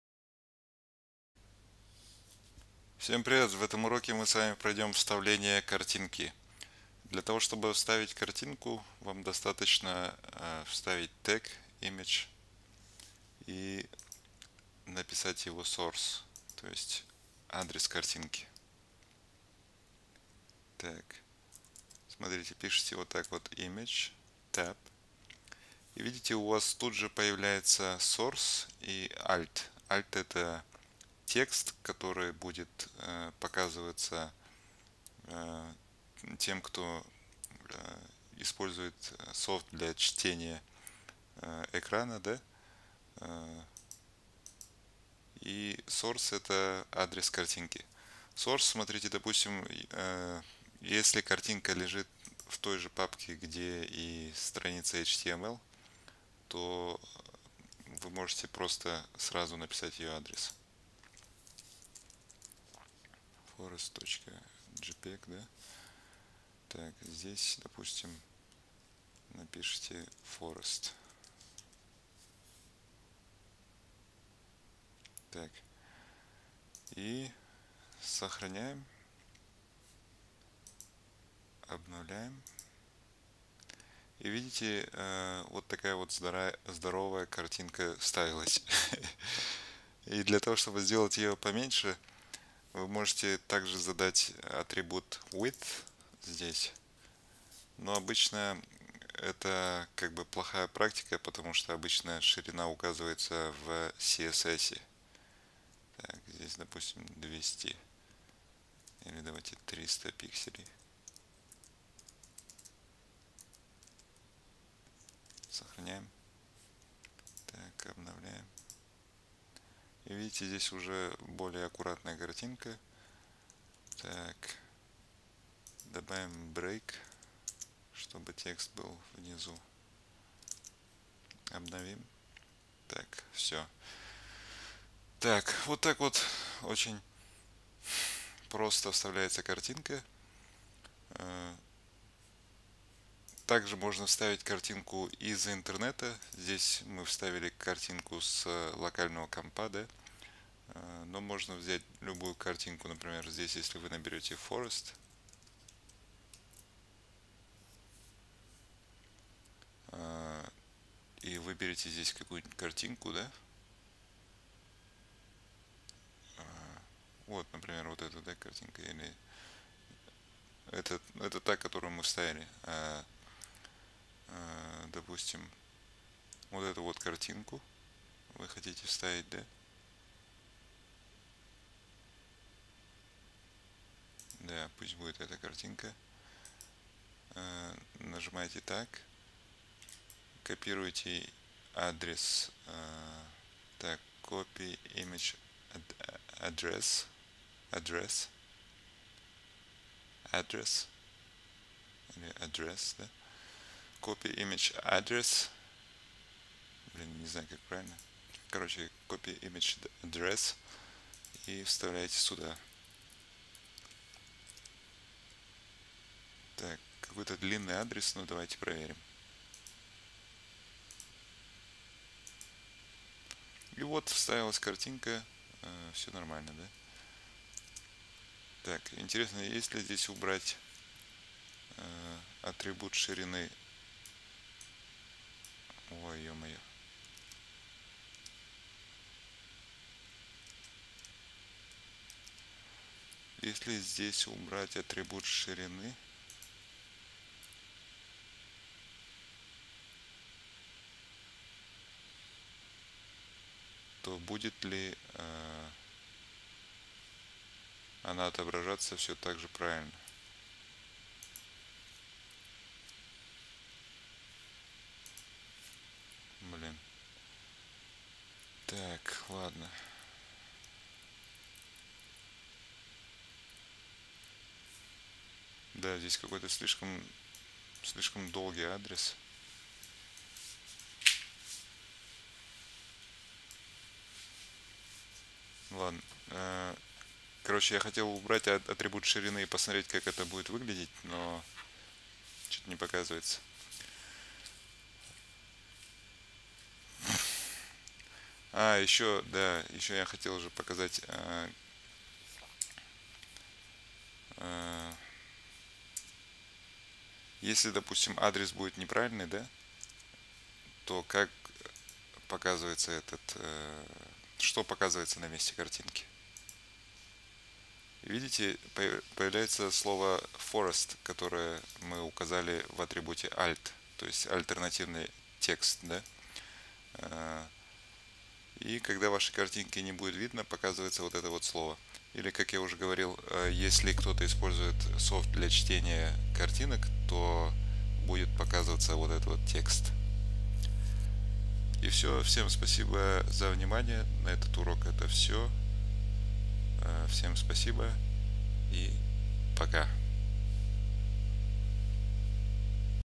Урок по Web development - 11 - Image Tag (Картинка)
HTML Image Tag  - img - тэг для вставки картинки или изображения в/на веб страницу.   :)